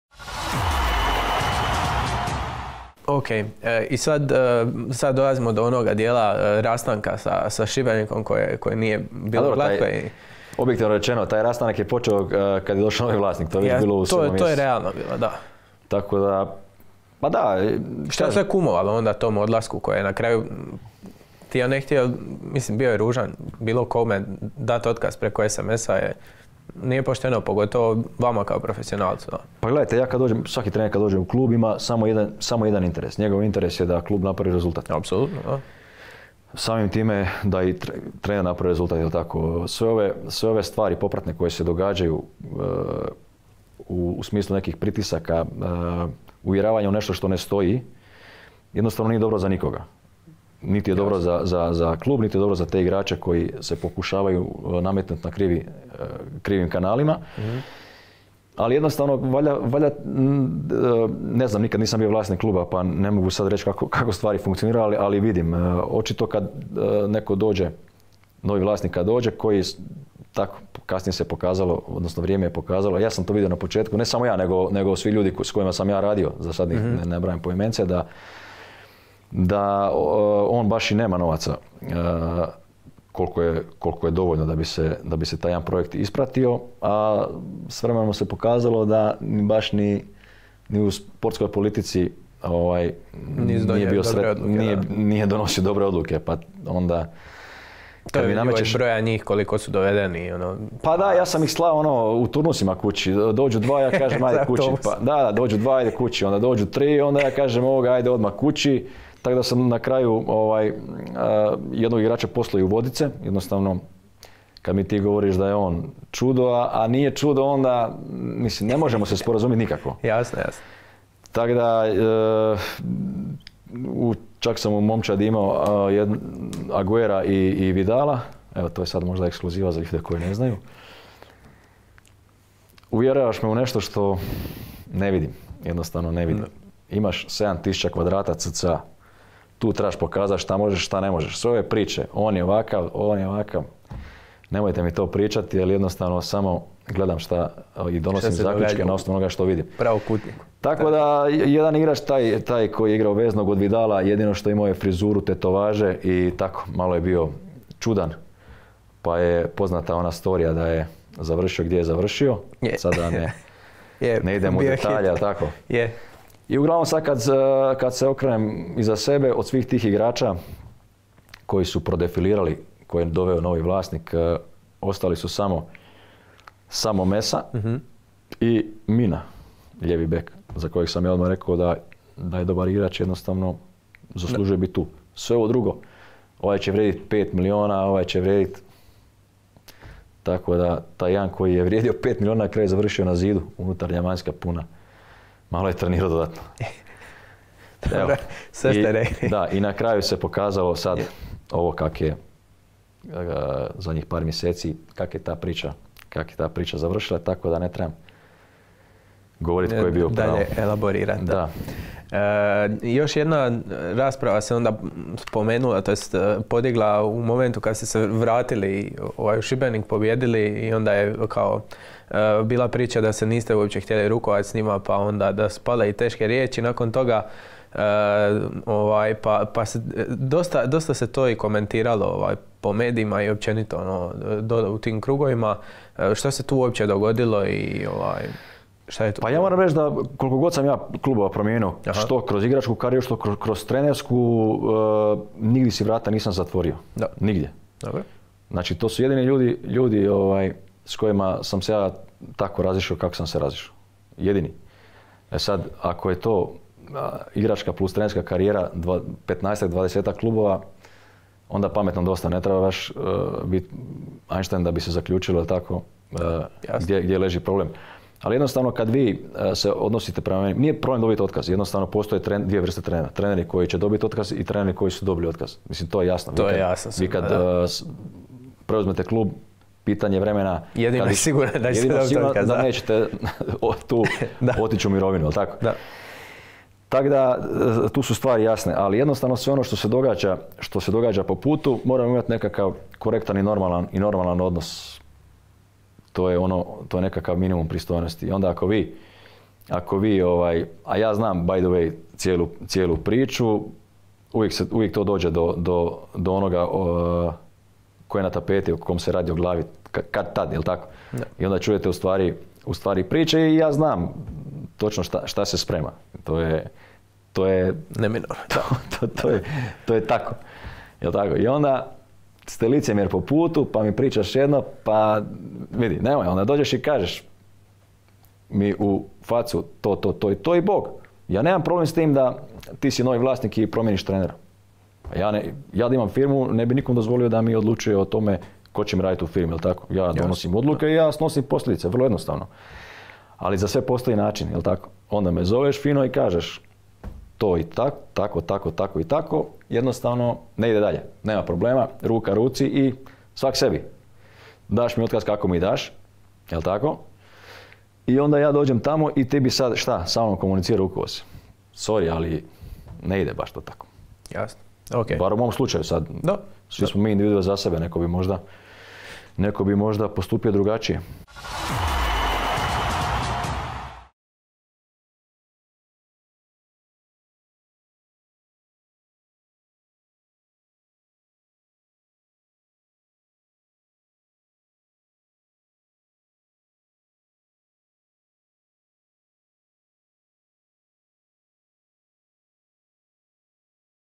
SuperSportske podcast S2E32 - Rendulić: "Vidio sam da on i nema novaca da bi se projekt ispratio."
U prošlom podcastu kod nas je gostovao Krunoslav Rendulić s kojim smo se dotakli njegovog angažmana na klupi u Šibeniku i odnosu s kolumbijskim vlasnicima.

Pratite nas na svim kanalima!

Ma kakav Valdas, Bjelica, Tomić - TI MOŽEŠ BOLJE!